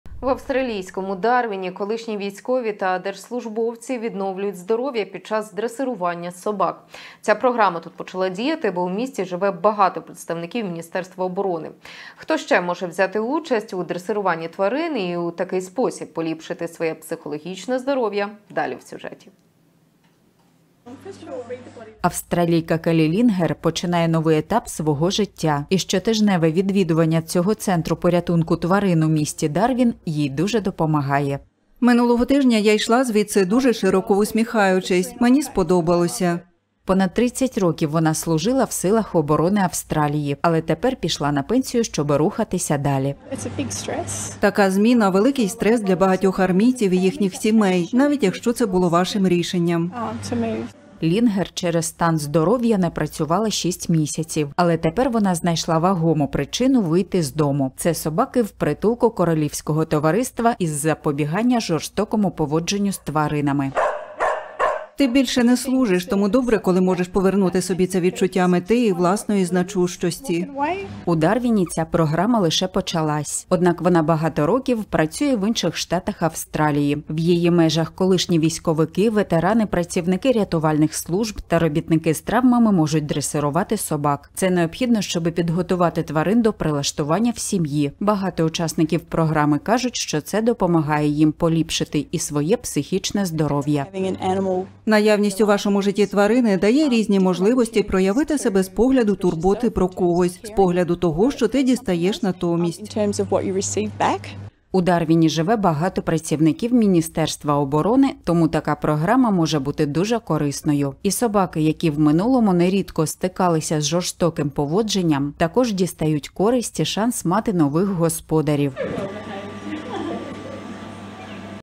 В Австралії колишні військові і держслужбовці відновлюють психологічне здоров’я, дресируючи собак
В австралійському місті Дарвін колишні військові та держслужбовці відновлюють здоров’я під час дресирування собак. Ця програма тут почала діяти, бо у місті живе багато представників міністерства оборони. Крім військових у програмі беруть участь працівники рятувальних служб та робітники з травмами можуть дресирувати собак. Це необхідно, щоб підготувати тварин до прилаштування в сім’ї. І собаки, які в минулому нерідко стикалися з жорстоким поводженням, також дістають користь і шанс мати нових господарів.